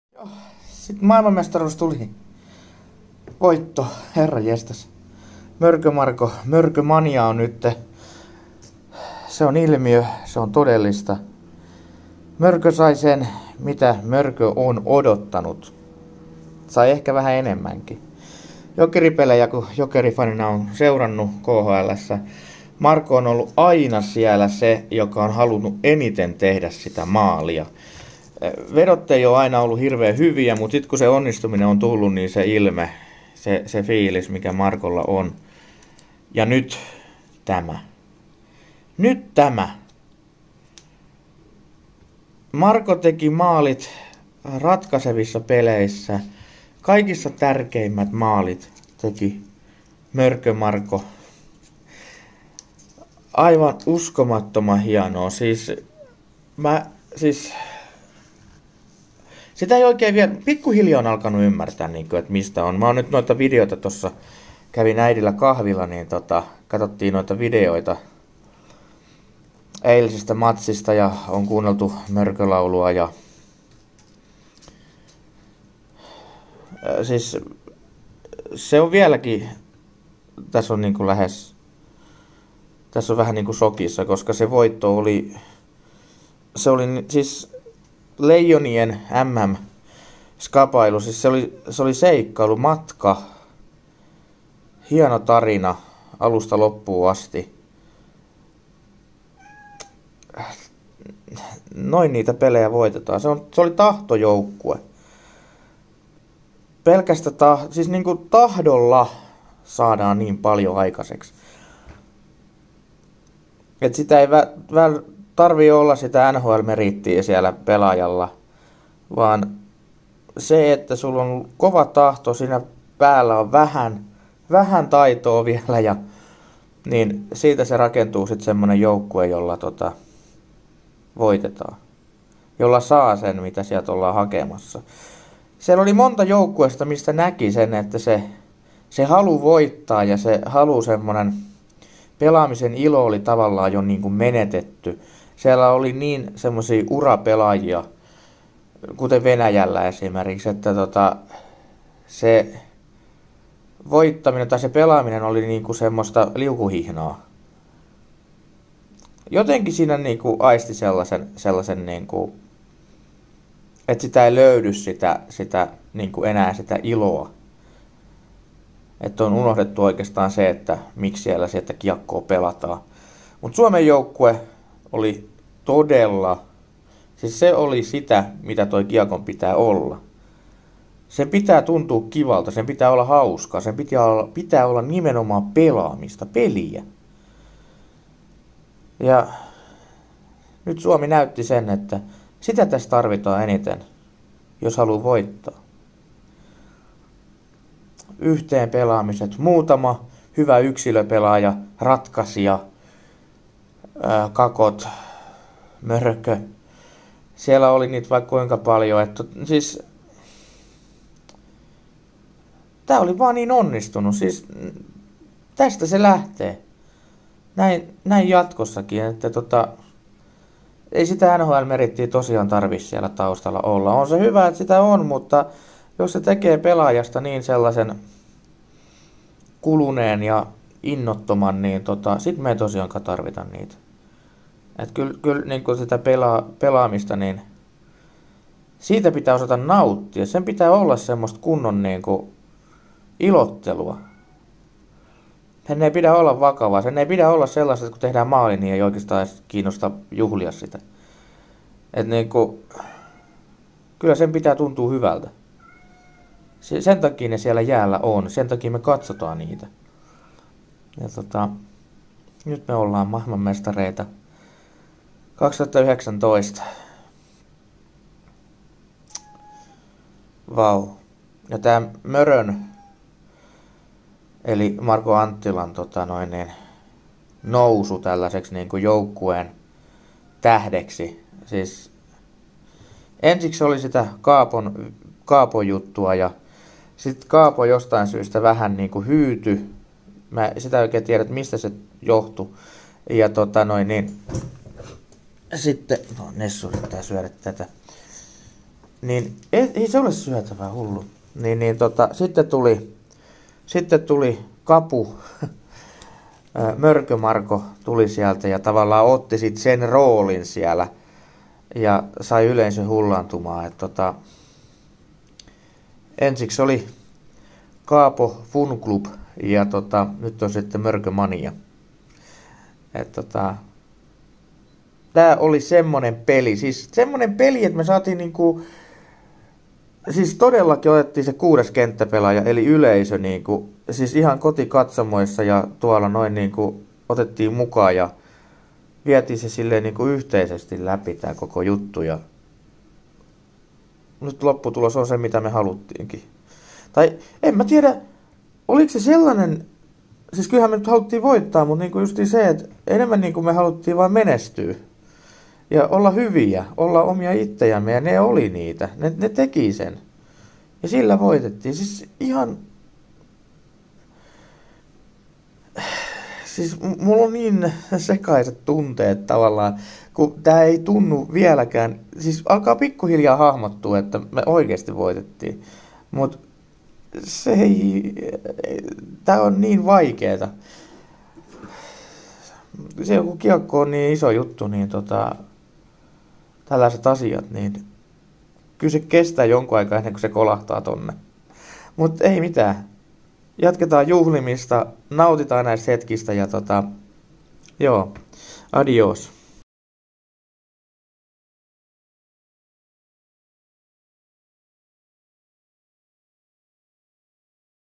Suomi on uusi maailmanmestari!!!!!
Nyt se tapahtui, joukkueella jolla on sydän ja tahto.
Suomi on uusi jääkiekon maailmanmestari!!